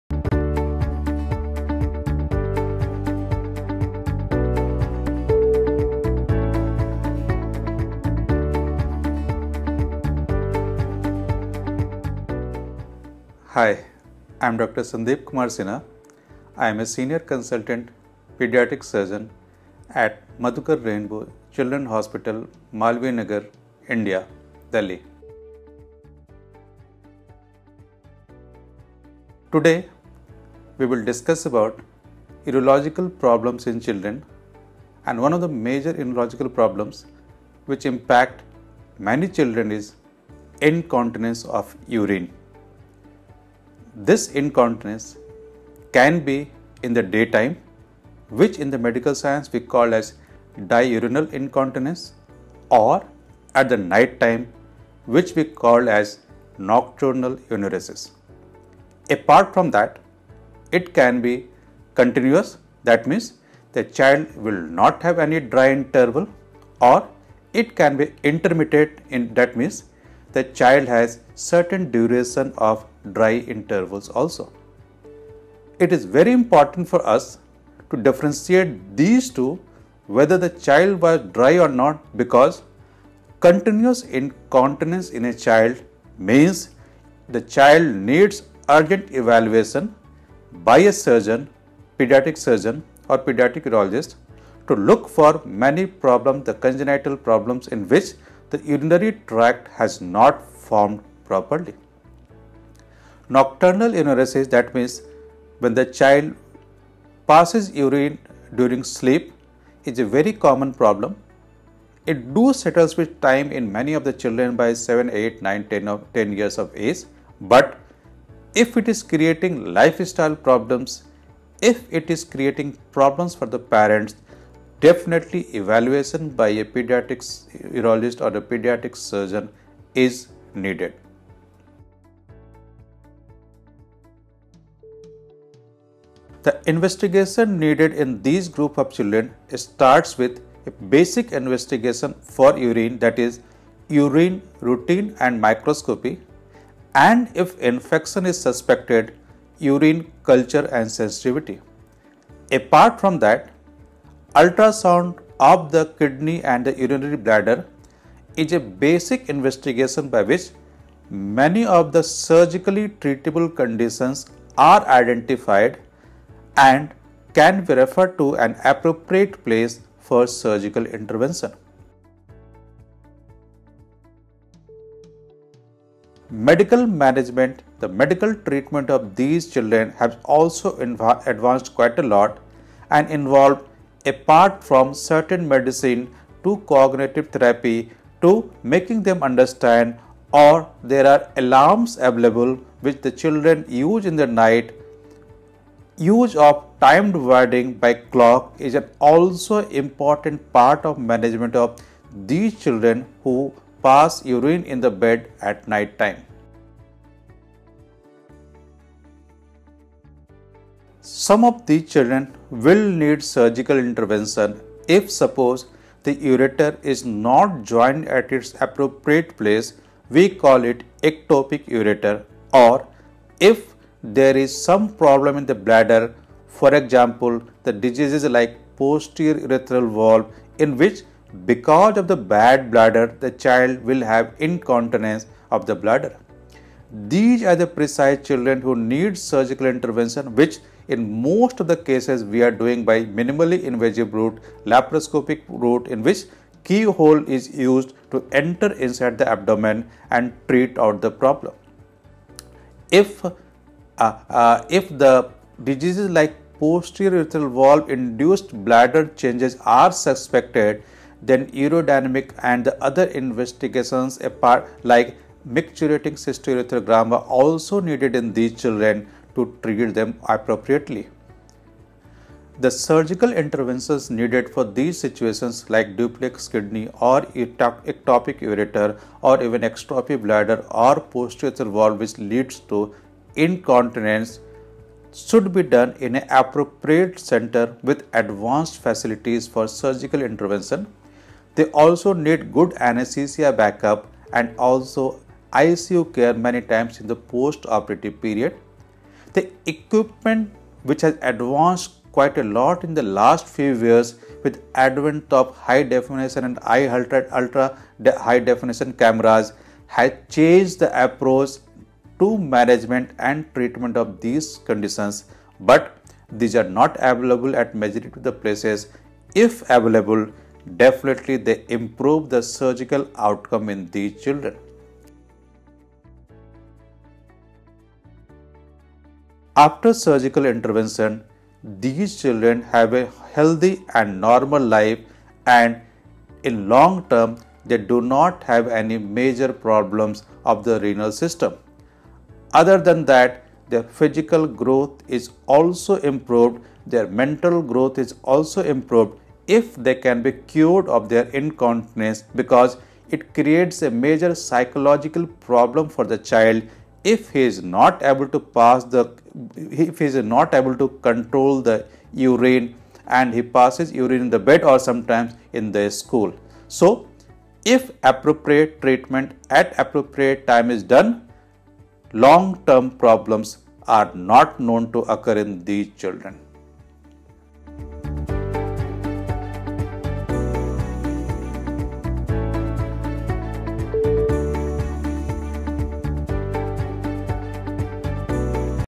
MedXpert Talks- Episode 5- Urinary Incontinence in Kids
Renowned Paediatric Surgeon Dr. Shandip Kumar Sinha is the Senior Consultant of Paediatric Surgery at Madhukar Rainbow Children’s Hospital, Malviya Nagar, Delhi, India.

In this video, he talks about Urinary Incontinence in children. Incontinence can be in the daytime, which is known as Diurnal Incontinence, or at night called Nocturnal Enuresis. It can be continuous as well as intermittent, depending upon its frequency. Such condition gets cured on its own by 7 to 10 years of age, but in case it persists, immediate evaluation by a Paediatric Surgeon is needed.

Urine Routine, Microscopy, Urine Culture, and Sensitivity are a few of the tests performed to detect infection in the bladder. Other investigation techniques include Ultrasound of the Kidney and the Urinary Bladder to ascertain whether surgical intervention is needed or not.
Medical Management of incontinence involves medications, Cognitive Therapy, keeping alarm clocks, etc. However, some of the medical conditions like Ectopic Ureter, Duplex Kidney, Exstrophy Bladder, and Posterior Urethral Valves (PUVs) require surgical intervention, which is done through minimally invasive methods. Preoperative check-up comprises of tests like Urodynamic and Micturating Cystourethrogram, which are done to chart out the appropriate plan of action. These surgeries should be executed in state-of-the-art medical facilities having proper anesthesia machines and intensive care units.
Post-surgery, these children lead normal life. Since Urinary Incontinence affects children psychologically as well, therefore, earlier intervention improves not only their physical health but also their mental health.

for information on treatments, doctors & hospitals in India.

Follow the link to know more about Dr. Shandip Kumar Sinha